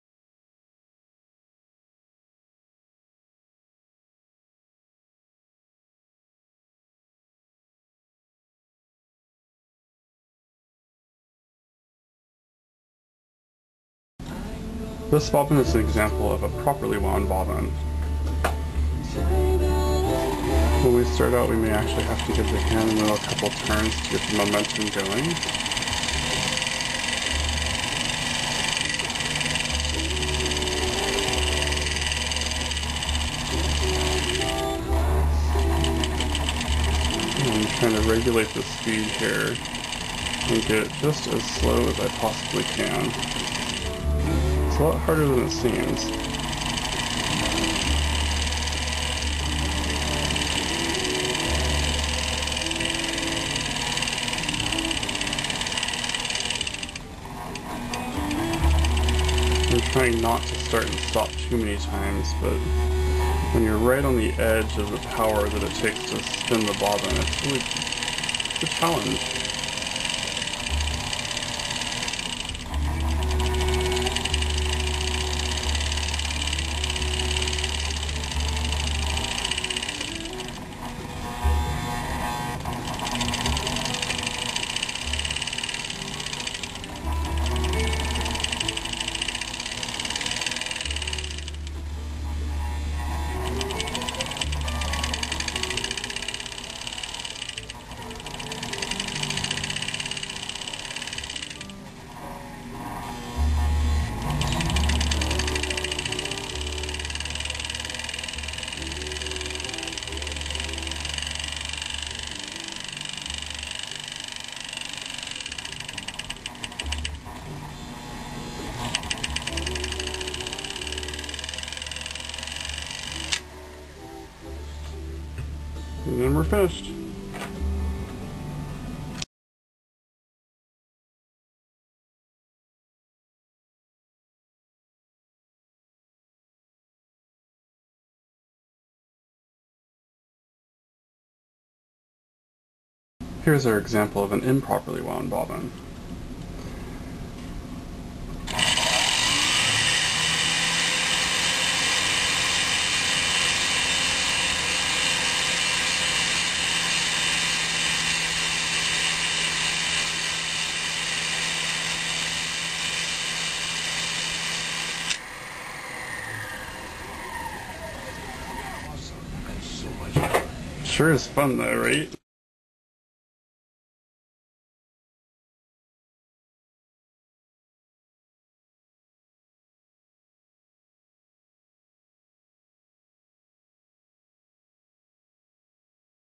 How to properly wind a bobbin
This movie show the proper and improper way to wind a sewing machine bobbin.  Winding a bobbin the wrong way will cause lots of problems with your stitch quality while you sew.  Learn more about bobbin winding and other sewing goodies